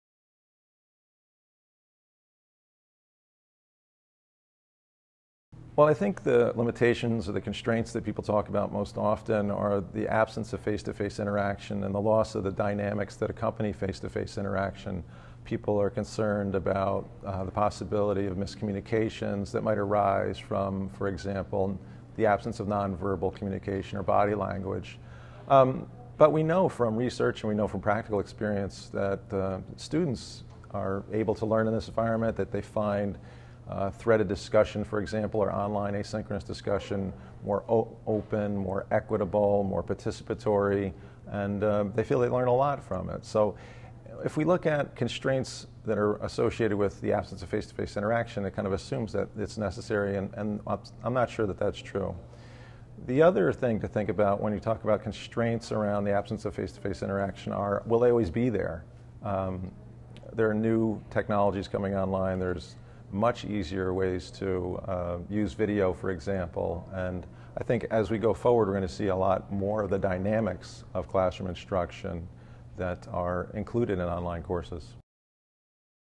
What Do You Lose in Teaching Online?
Peter Shea discusses the constraints of learning online.

This video is associated with the Open SUNY Interested in Online Teaching course and is featured in the online teaching readiness assessment

Feel free to use and share these resources with anyone interested in learning more about online teaching and learning.

Access to this video is provided by Epigeum, part of Oxford University Press.